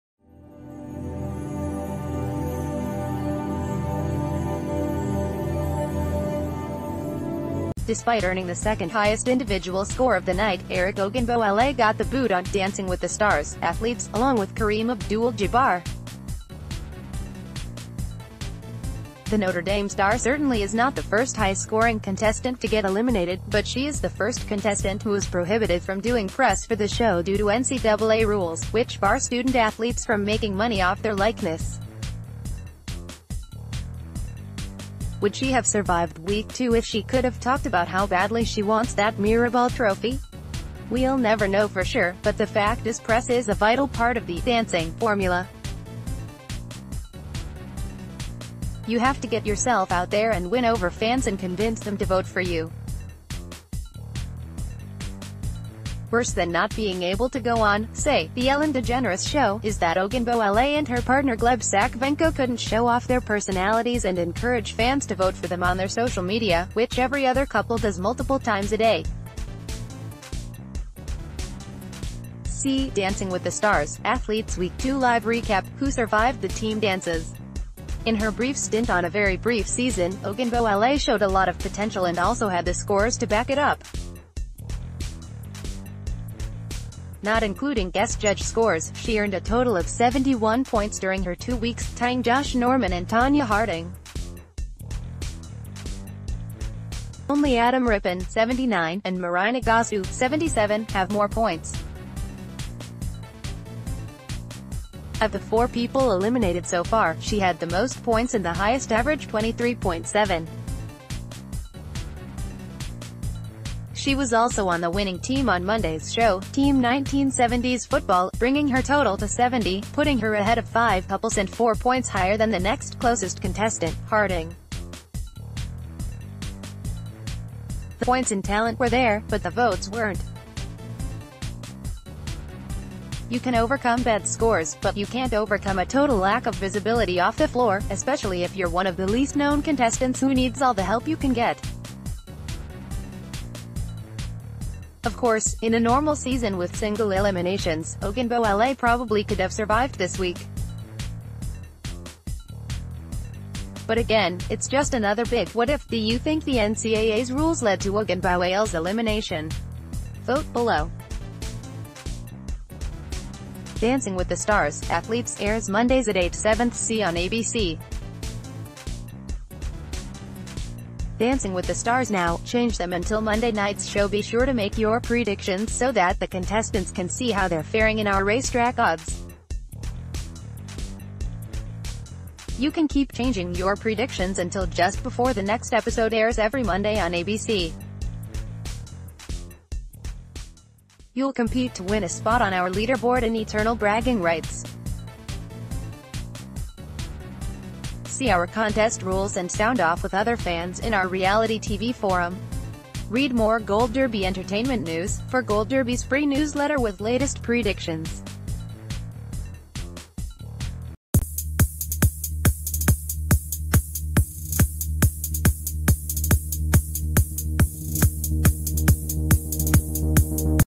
Breaking News | Did NCAA rules doom Arike Ogunbowale on ‘Dancing with the Stars: Athletes’? [POLL]
Despite earning the second highest individual score of the night, Arike Ogunbowale got the boot on “Dancing with the Stars: Athletes,” along with Kareem Abdul-Jabbar. The Notre Dame star certainly is not the first high-scoring contestant to get eliminated, but she is the first contestant who was prohibited from doing press for the show due to NCAA rules, which bar student athletes from making money off their likeness. Would she have survived Week 2 if she could’ve talked about how badly she want...